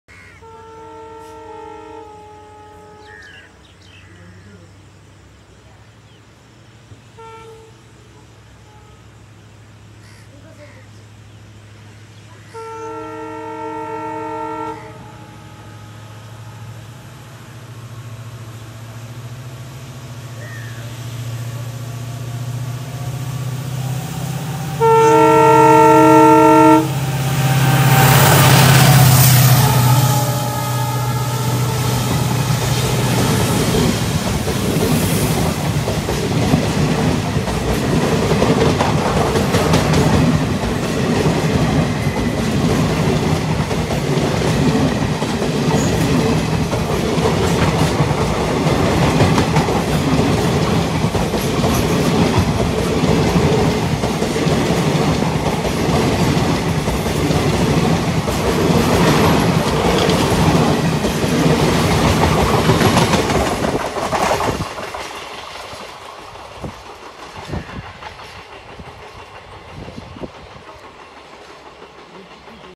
Visakhapatnam Mumbai LTT Express With Gooty WDP 4D Loco at Dosapadu,Gudivada | Boarded Bidar MTM EXP
Honking Gooty WDP4D with Visakhapatnam Mumbai LTT Express taken from Bidar Machilipatnam Express || Indian Railways

Gooty WDP4D with Visakhapatnam MumbaiLTT Express